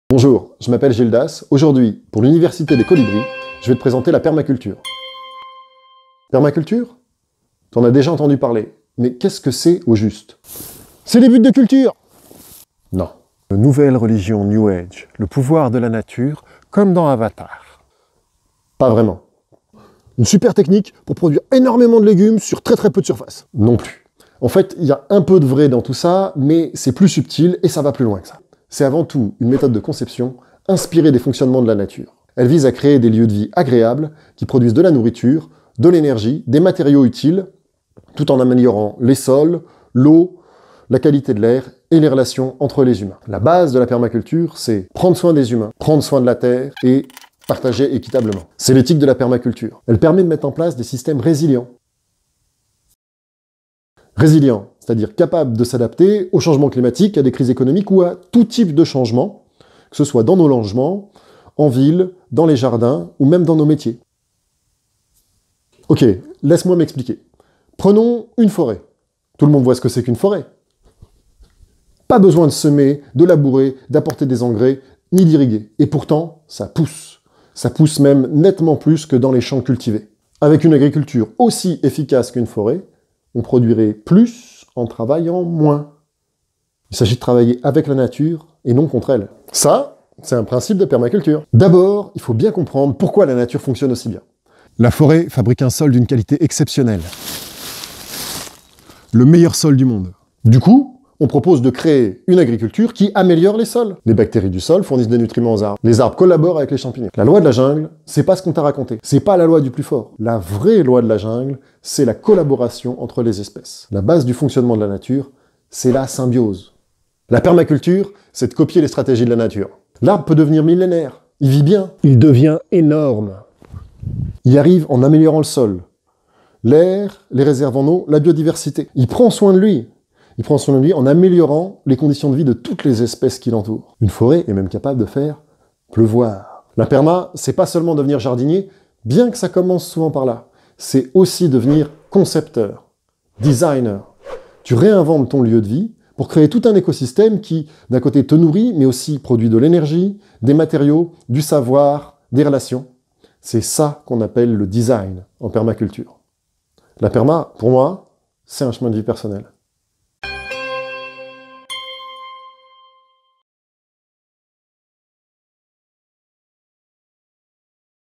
La Permaculture ce n'est pas...
La Permaculture, c'est quoi ? Petite explication en 3' de Gildas Véret, avant de commencer le Mooc "Conception en Permaculture", formation en ligne gratuite de Colibris !